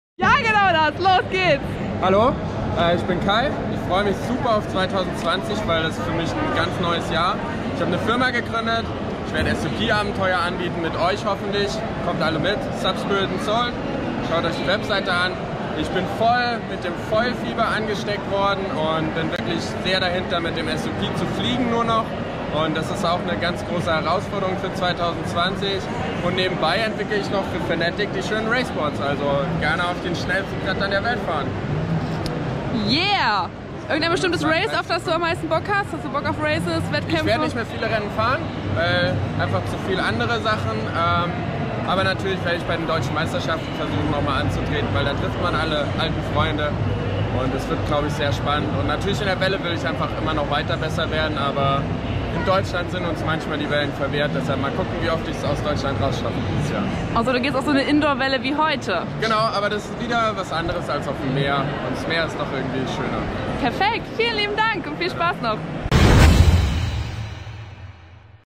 Step Inside – Kai Steimer auf der Boot Düsseldorf 2020
Kai-Nicolas Steimer ist Fanatic Teamfahrer, ein Urgestein der deutschen SUP Race Szene, Shaper und betreibt seit diesem Jahr mit SUPSpirit Soul einen SUP Reiseanbieter